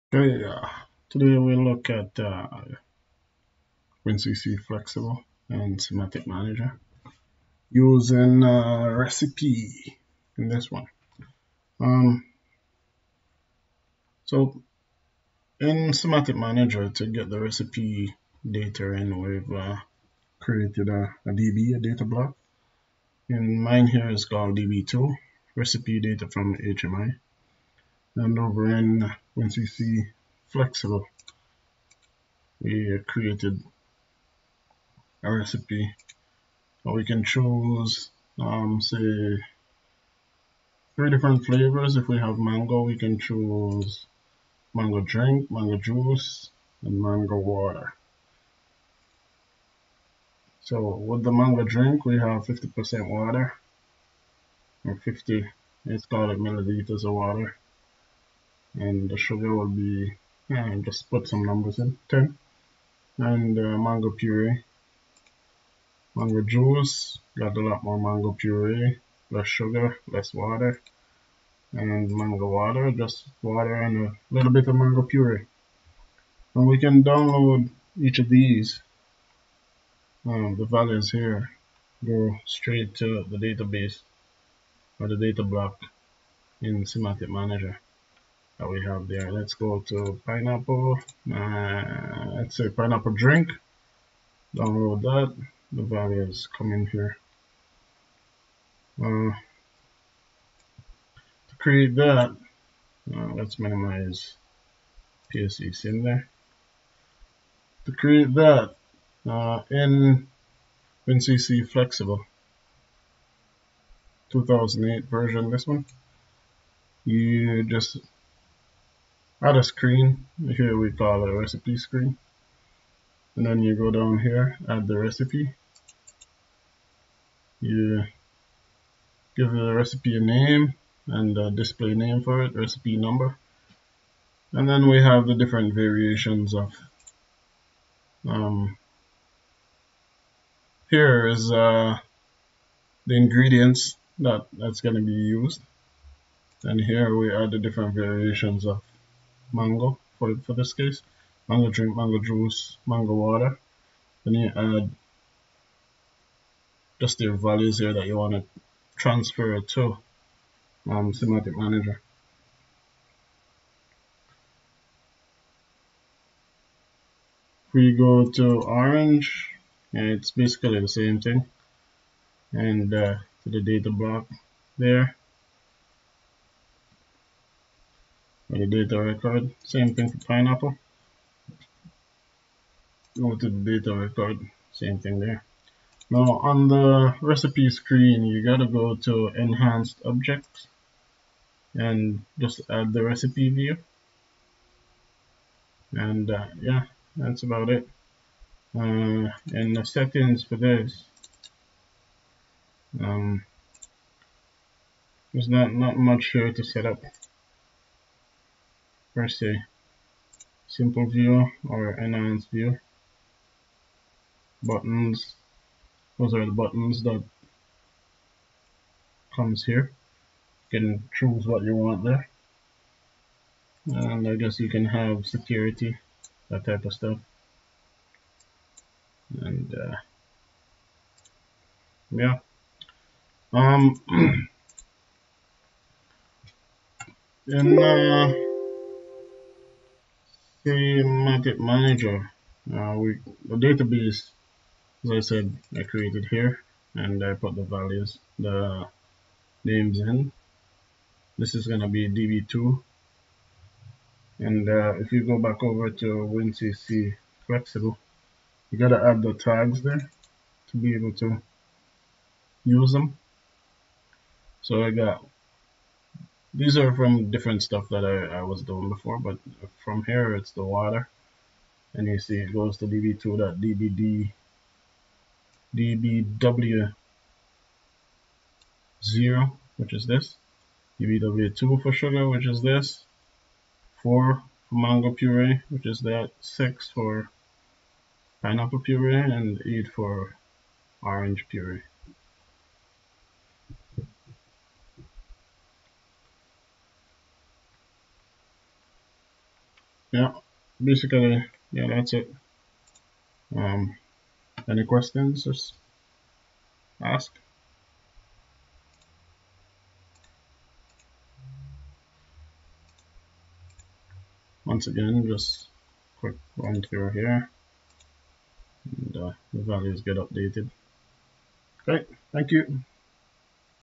WinCC Flexible Recipes to Simatic Manager DB
In this video creating recipes in WinCC Flexible 2008 and downloading the values to a Simatic Manager DB is demonstrated.